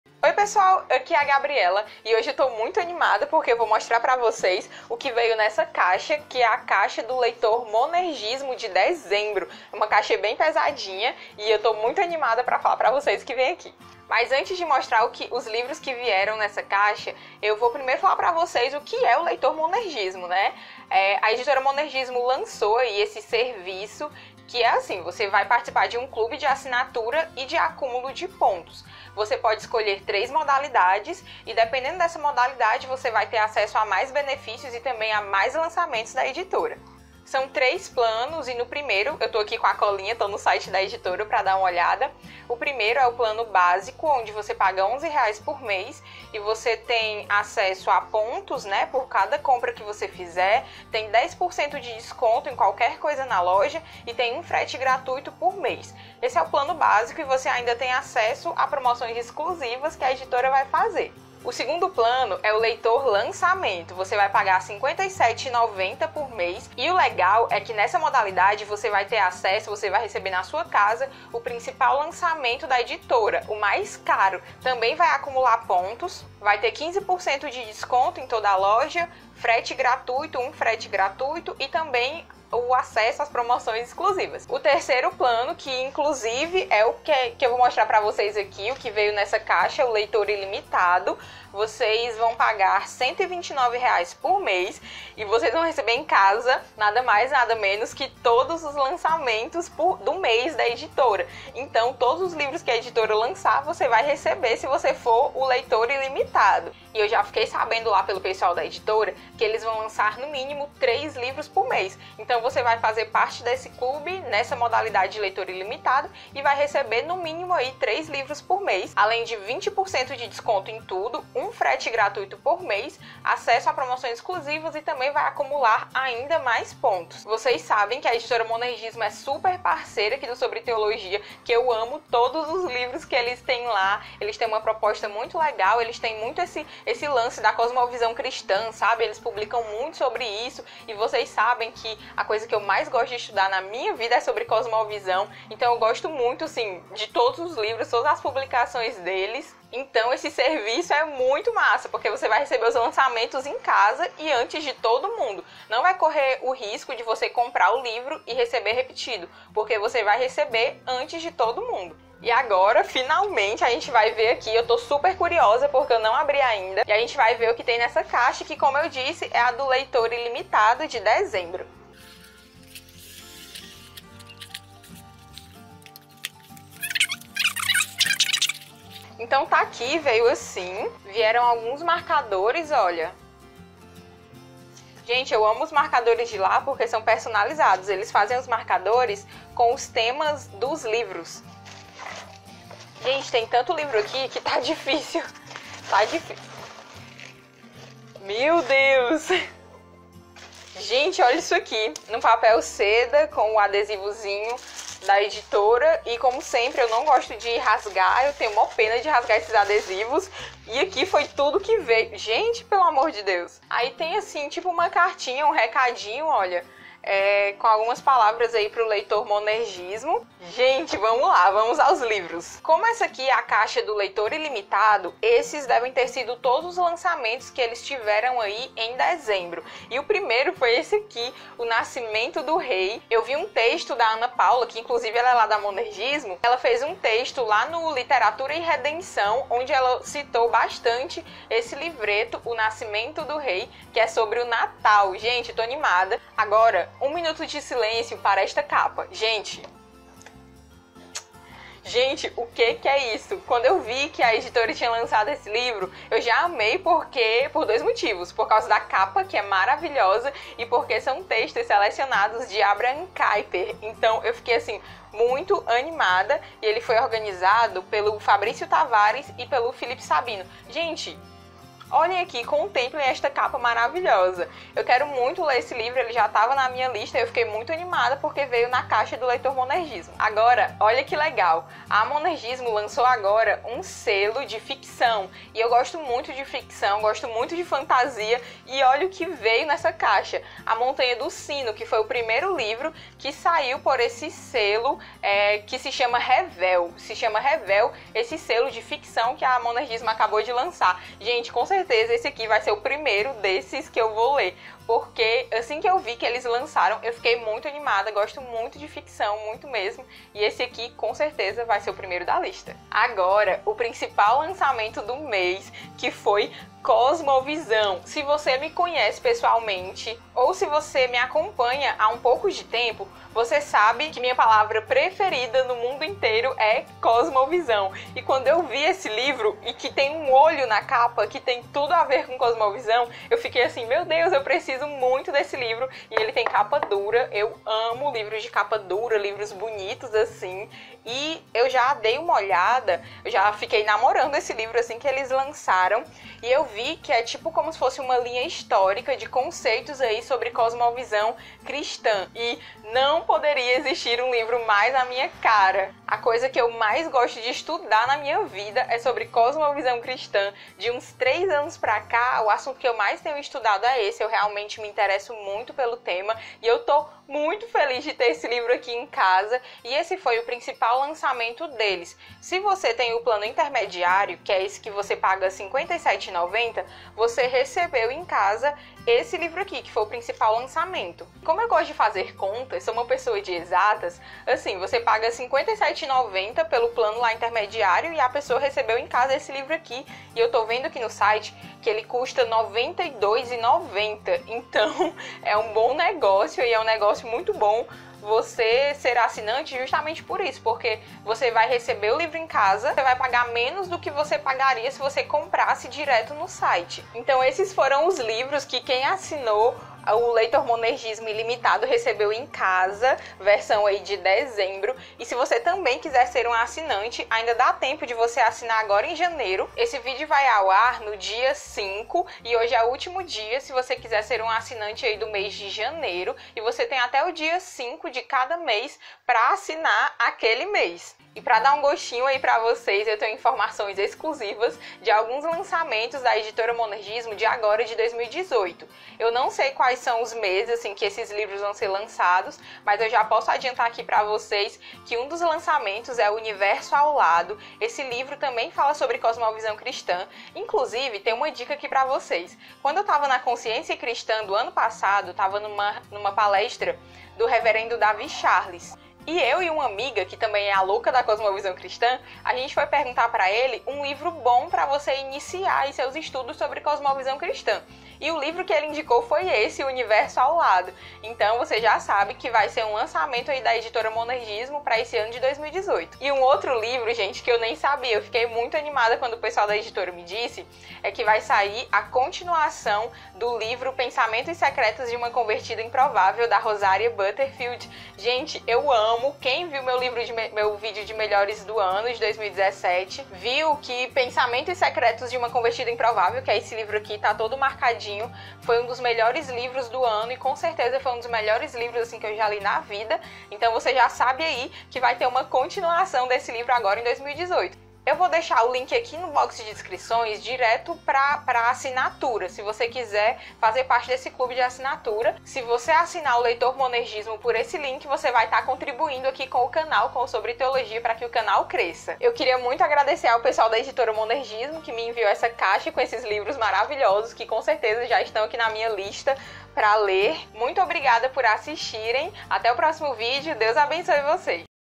\\unboxing: abrindo a caixa do LEITOR MONERGISMO ILIMITADO
No vídeo de hoje mostro pra vocês uma caixa muito legal que recebi da Editora Monergismo: a caixa do Leitor Ilimitado.

Link para assinar o Leitor Monergismo(assinando por aqui você ajuda o canal)